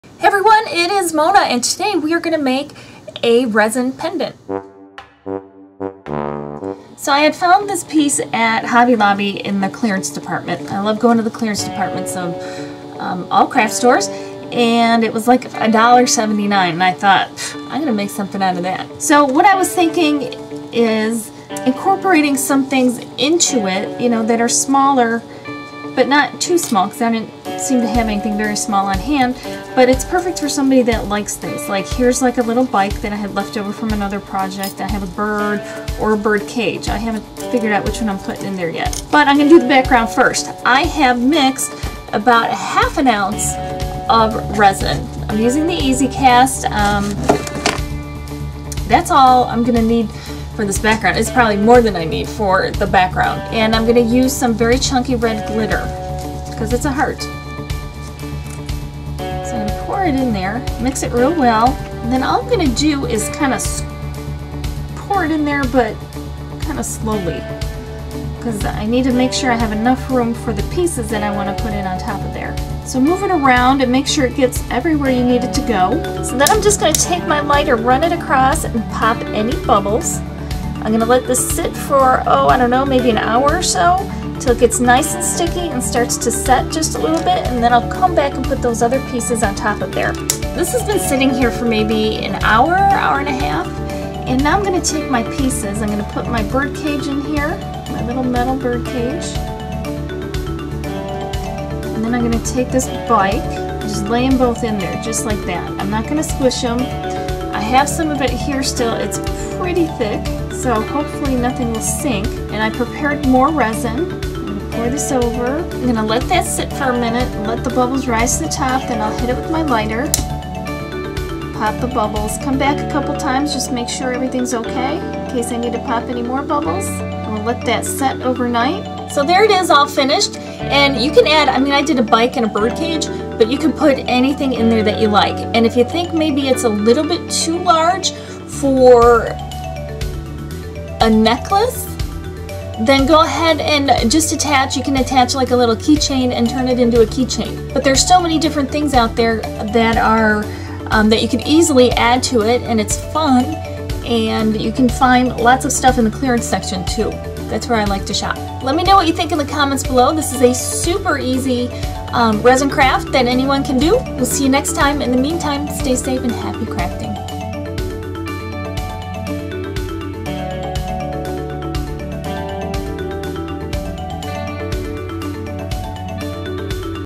Resin Heart Pendant DIY
This is a super easy resin craft.  Use some glitter, resin, and small things to add to make it personalized.  This is a big bezel that could be used as a pendant or a keychain!  Watch the DIY craft tutorial to see how I did it!

AND you can find the full directions and material

Mona